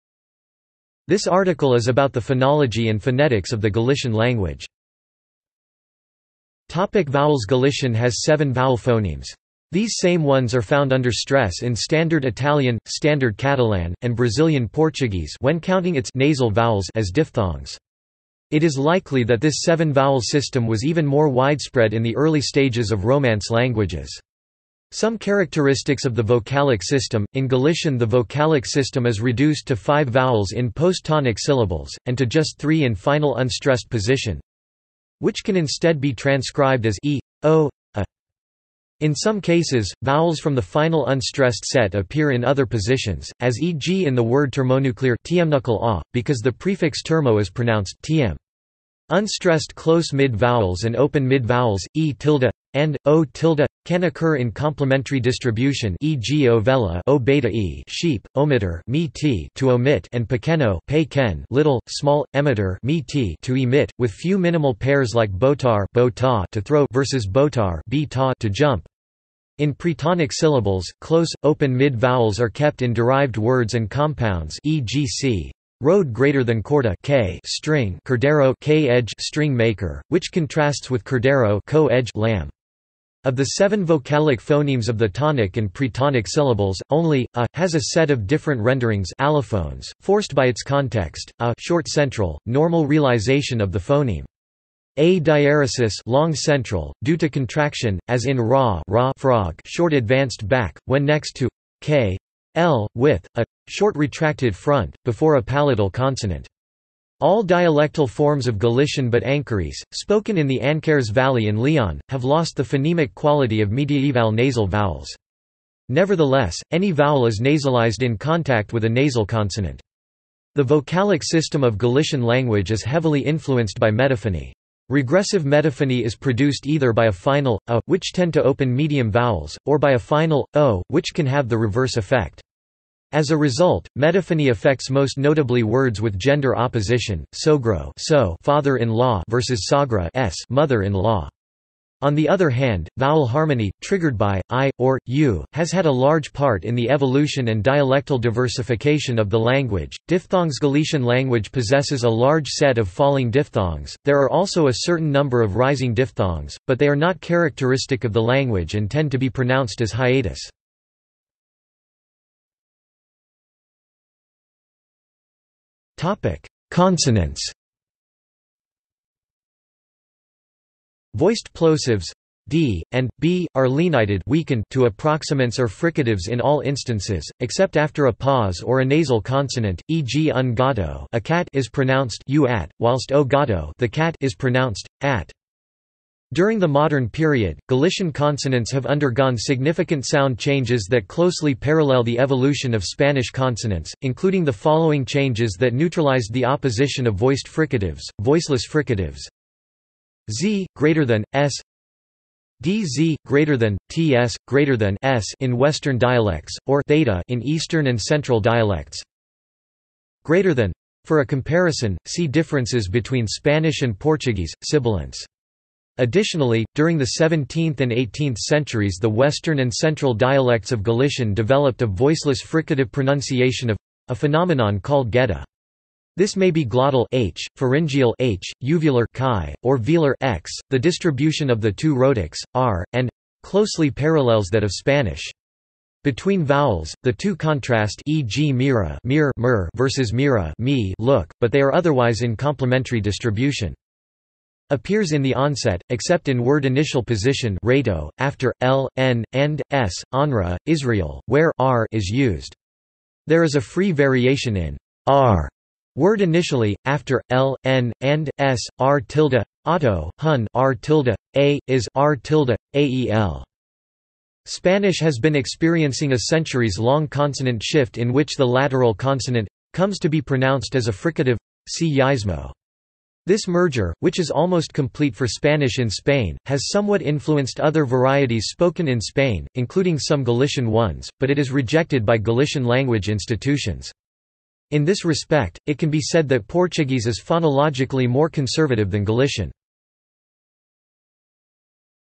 Galician phonology | Wikipedia audio article
This is an audio version of the Wikipedia Article:
Galician phonology

00:00:06 1 Vowels
00:02:42 2 Consonants

- Socrates

SUMMARY
This article is about the phonology and phonetics of the Galician language.